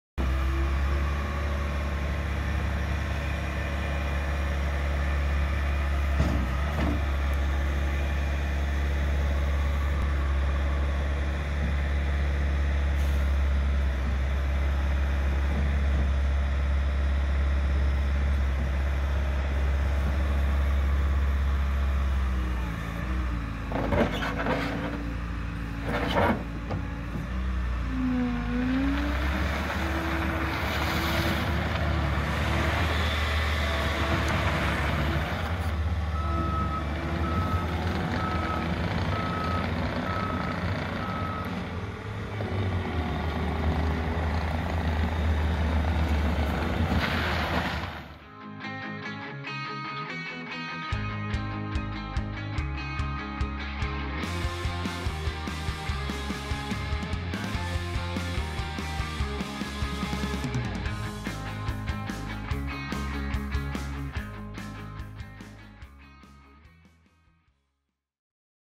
2021 Bobcat T76 Skid Steer Track Loader
Watch this 2021 Bobcat T76 Skid Steer Track Loader at work!
S/N: B4CE14146
4-Cyl Turbo Diesel 2-Speed
EROPS, High Flow, Hydraulic Quick Coupler, Auxiliary Hydraulics, 74" GP Loader Bucket, A/C, Heat, AM/FM/BT, Electric Port, Control Selector, Touch Screen Monitor, Rear Facing Camera, 2,900LB Operating Capacity, Joysticks Controls, Work Lights, 13" Rubber Tracks
Hours: 205